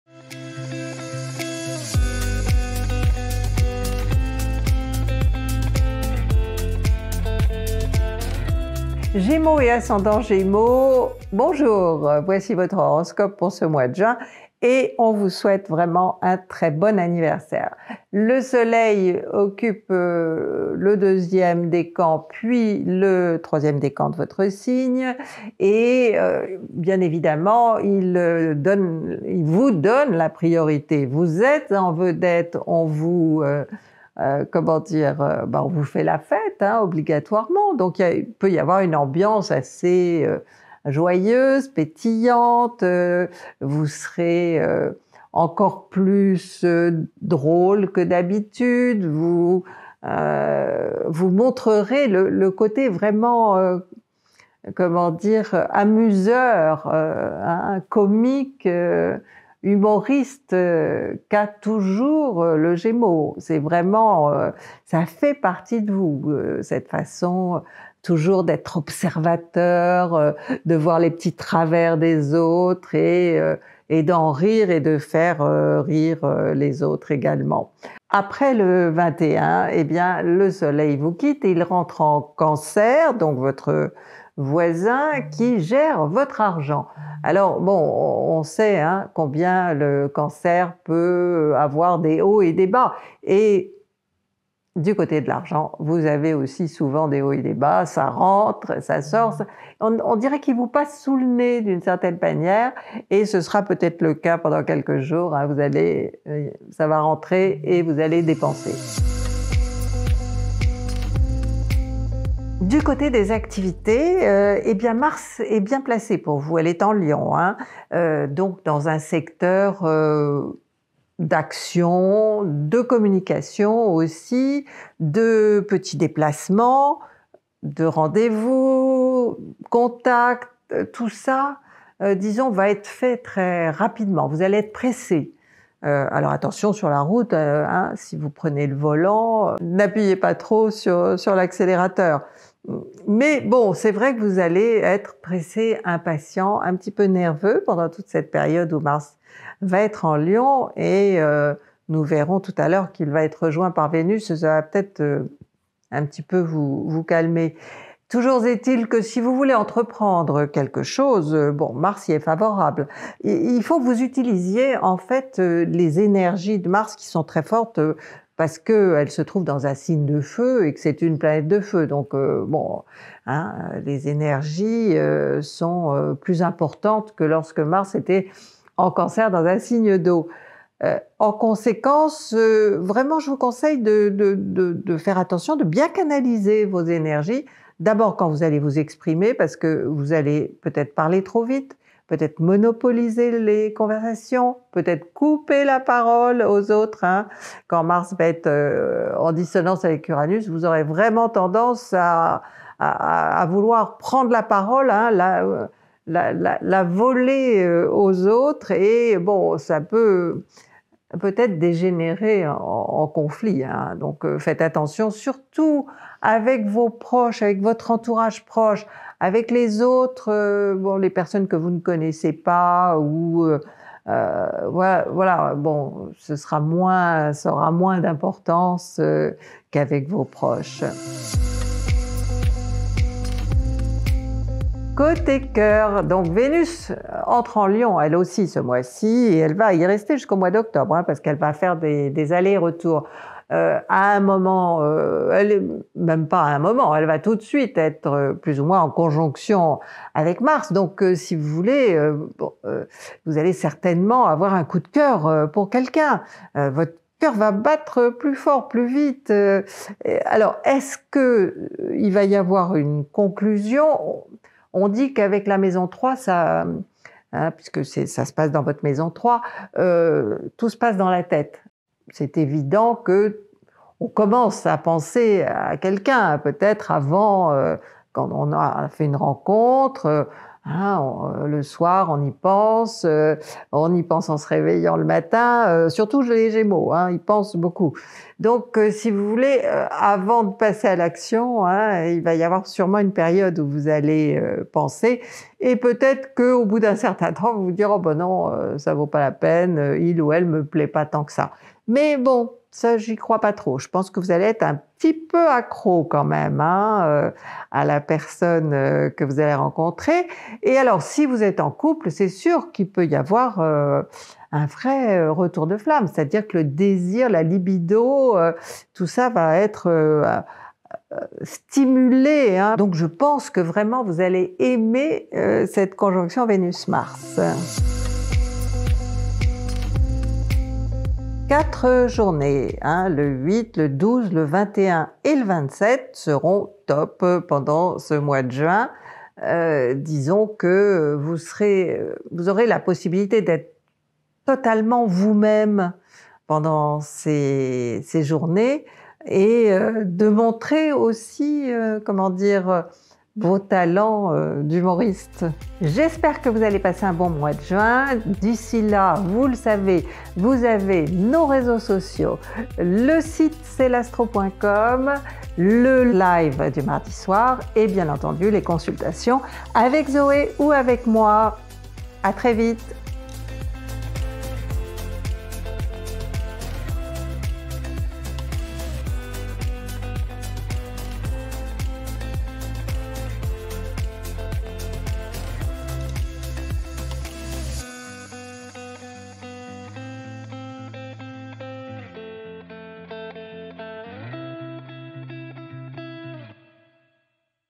Horoscope Gémeaux ♊️ Juin 2023 par Christine Haas
🌞 horoscope gratuit du signe du gémeaux pour le mois de juin #2023 avec des conseils en astrologie et en psychologie.

⭐️ Retrouvez votre signe:
0:00 Introduction
0:08 Ambiance
1:48 Activités
3:51 Vie affective
5:27 Vos jours top
5:55 Bye bye